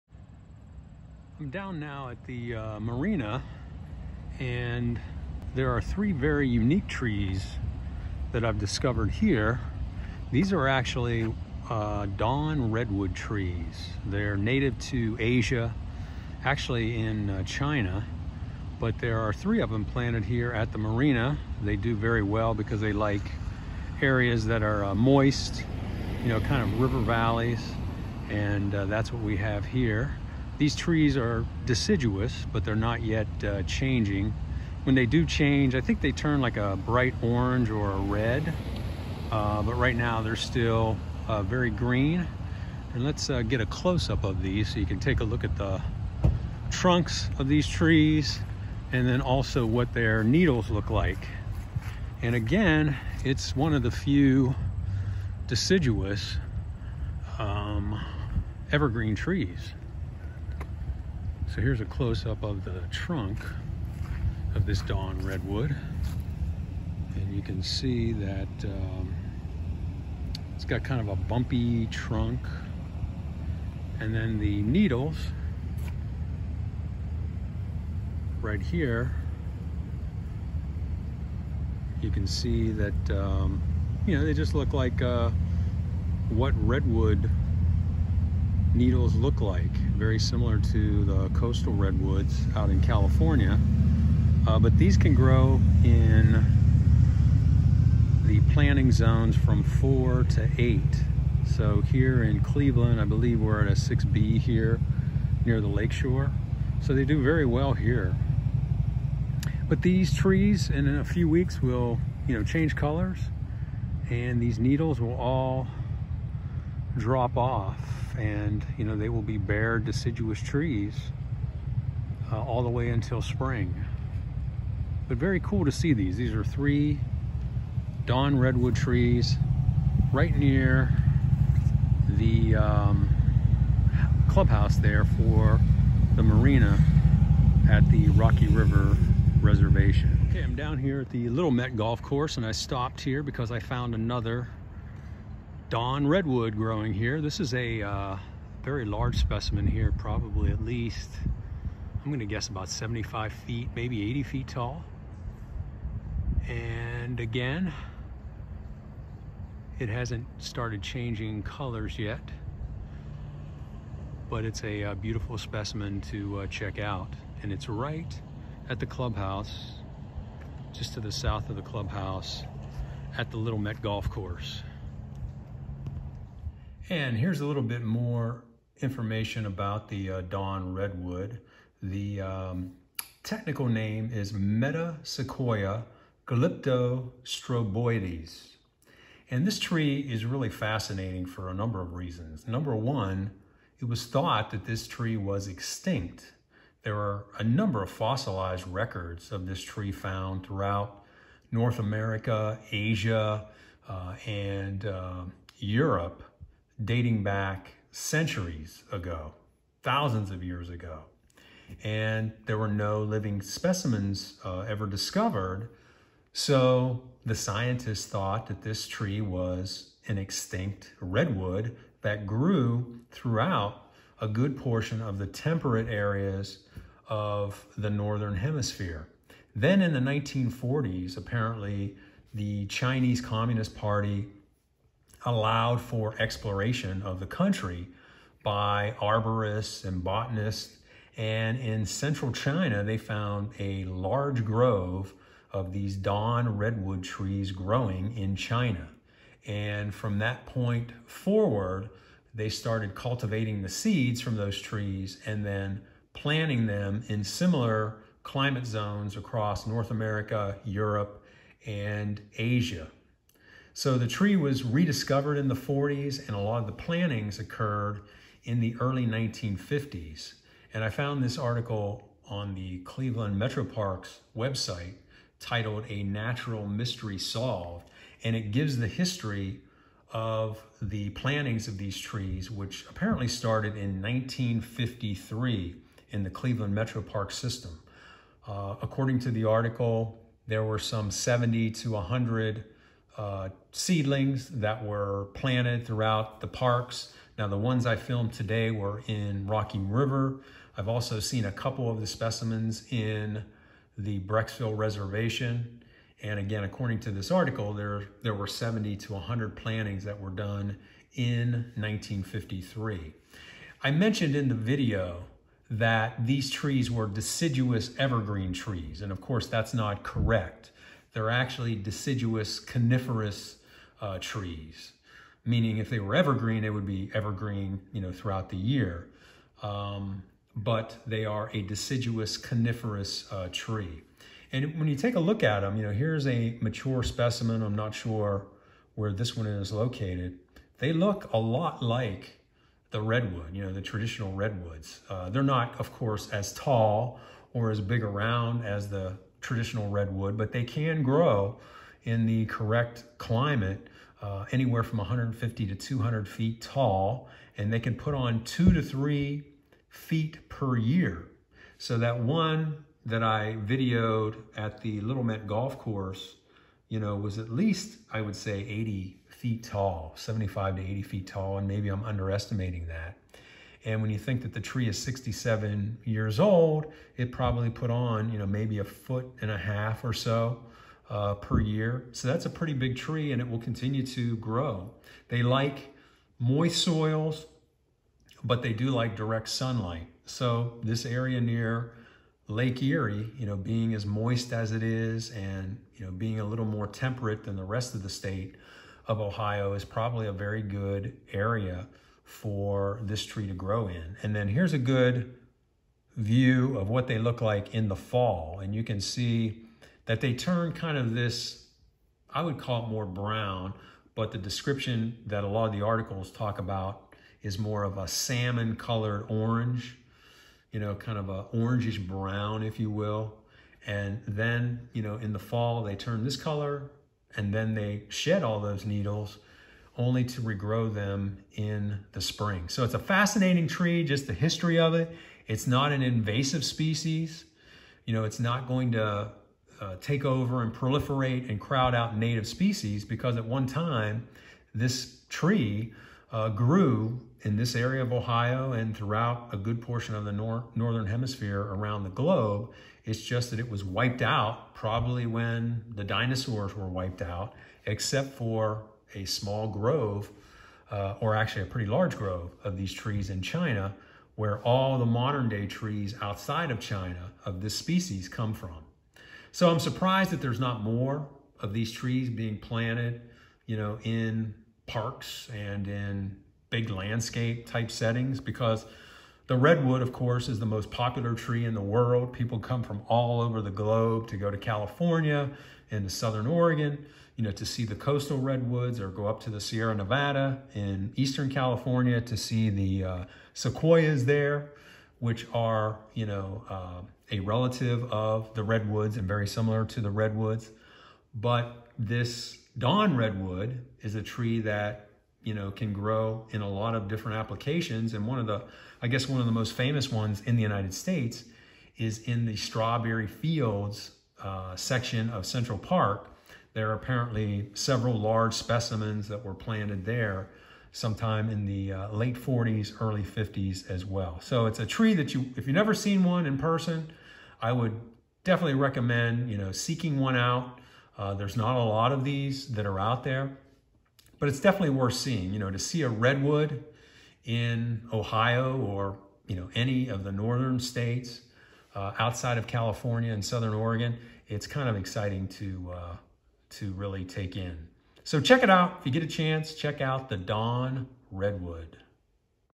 Yes, Redwood Trees do grow in Ohio!
The majestic Dawn Redwood Tree is thriving in Northeast Ohio. Once believed to be extinct, this species was rediscovered in China in the 1940s and have since been transplanted across the globe. The oldest trees in the Cleveland area were planted in the early 1950s.

If you enjoyed watching this video, you may also want to check out these videos I've done featuring Dawn Redwood trees: